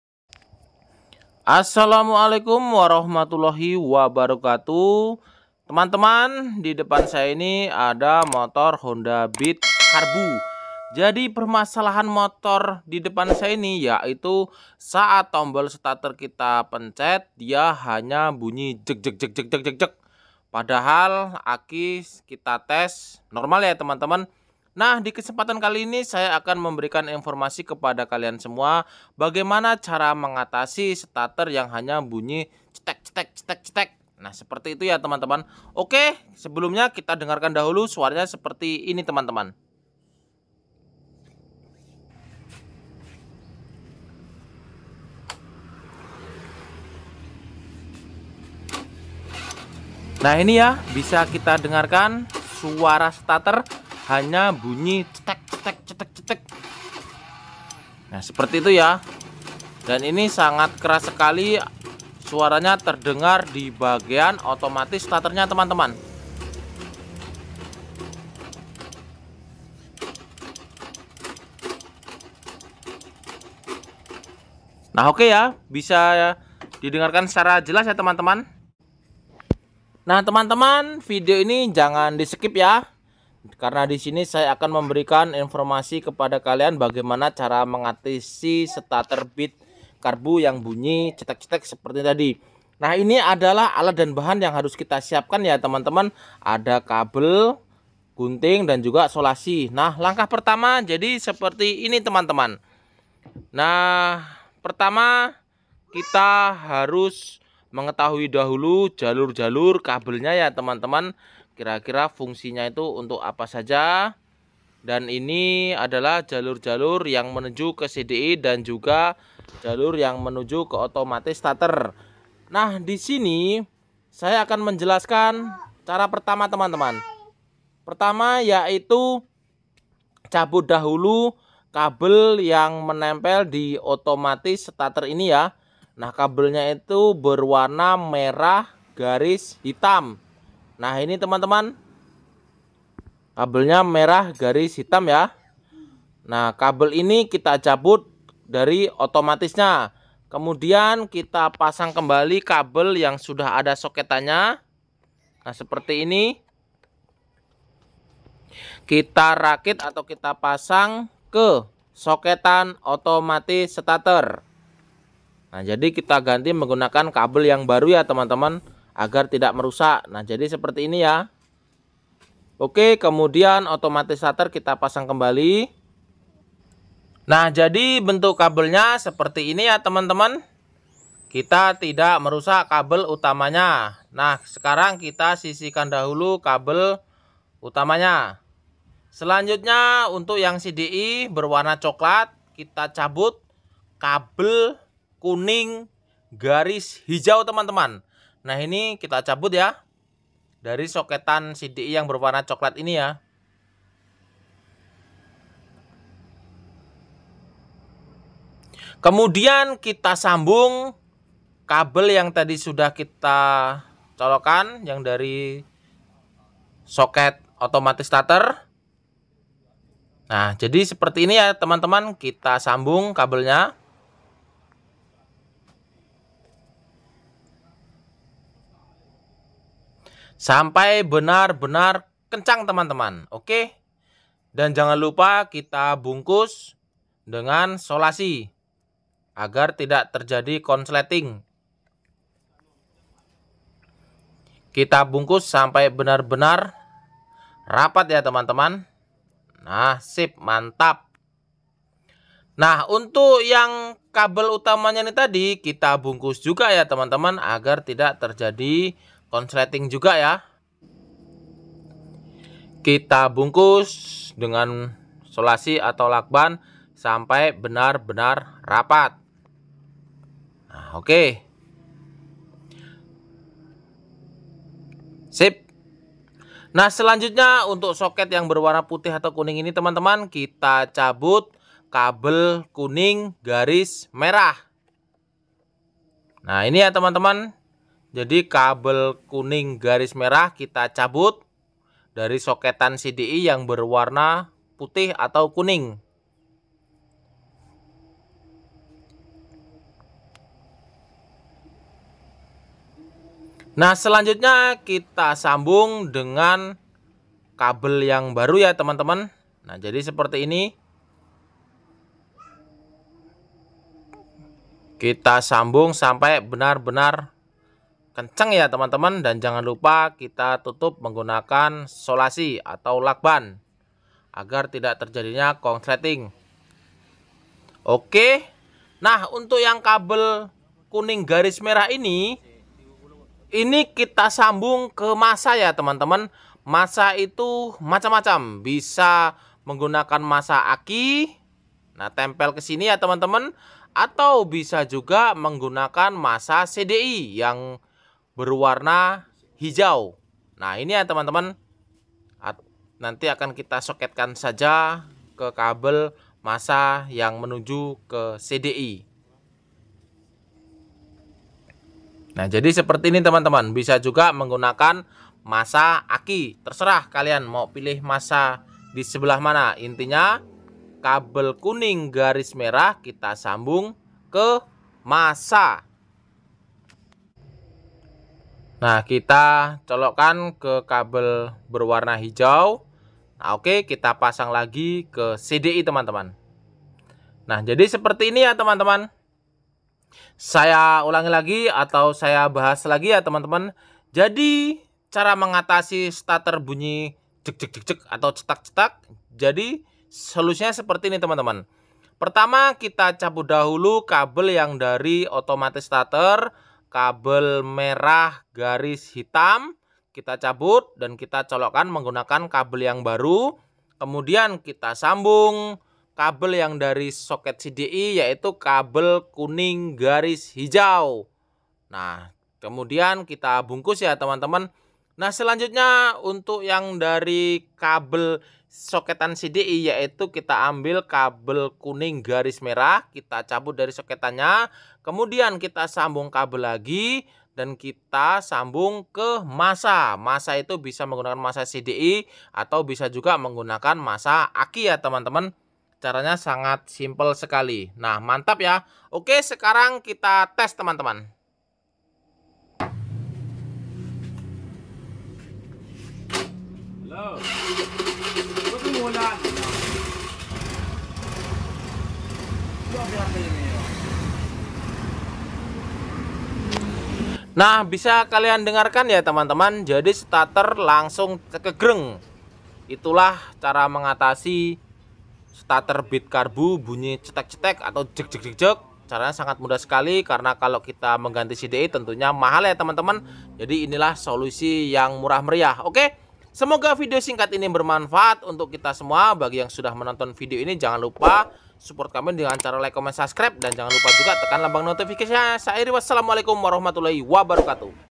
Solusi Cara Mengatasi Stater Beat Karbu Bunyi Cetak Cetak, Jek Jek Jek, Ikuti Tutorial Ini Part 001
Setater beat Karbu bunyi cetak cetak seperti aki soak ngedrop padahal sumber utama bukan dari aki, masalah yang sering terjadi ada dibagian CDI Dimana relay stater lemah, SOP ny wajib ganti CDI baru akan tetapi tentu mahal,,

di kesempatan kini saya akan memberikan informasi bagimana car mengatasi stater beat kar Karbu bunyi cetak cetak, pertama putus kabel relay stater kabel warna merah garis hitam, sambung dengan kabel kuning garis hijau yang menuju CDI,,

selanjutnya Kabel dar CDI warna kuning garis merah disambung dengan masa aki atau masa CDI